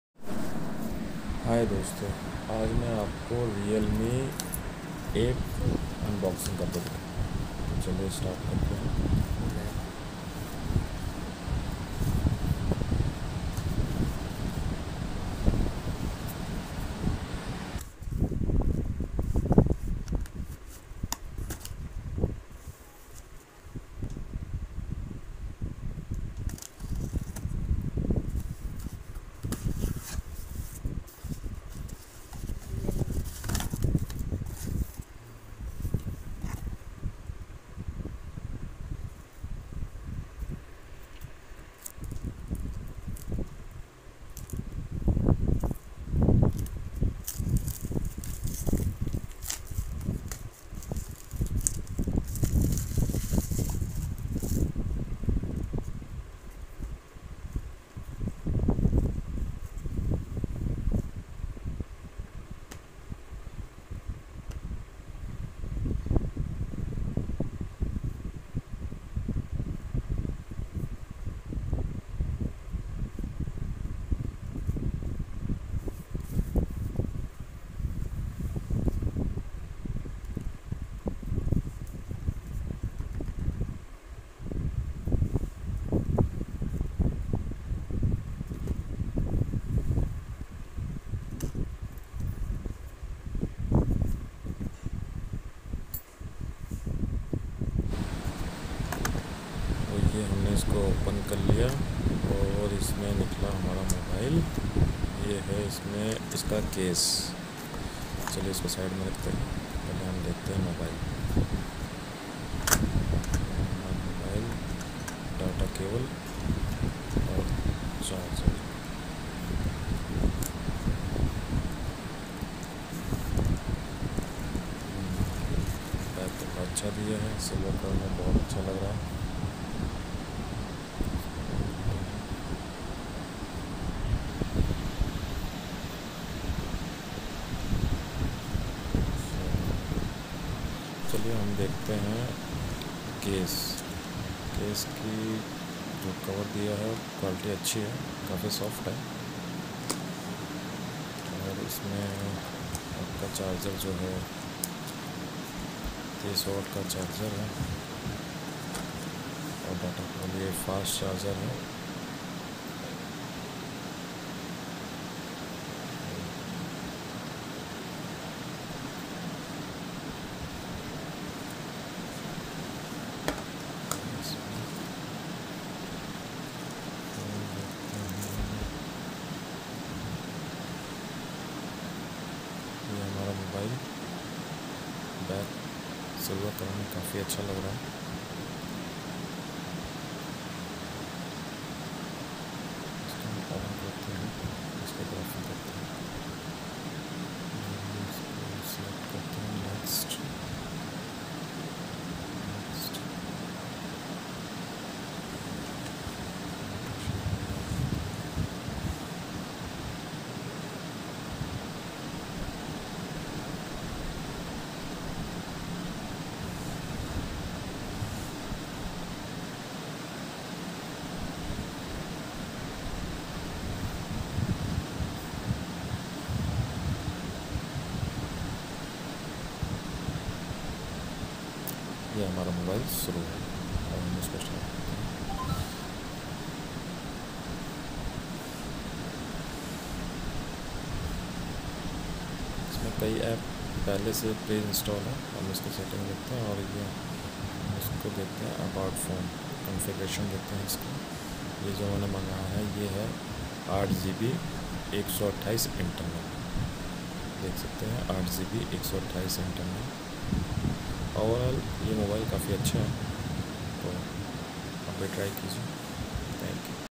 realme 8 unboxing
nice phone good performance
phone backup 2 days
camera good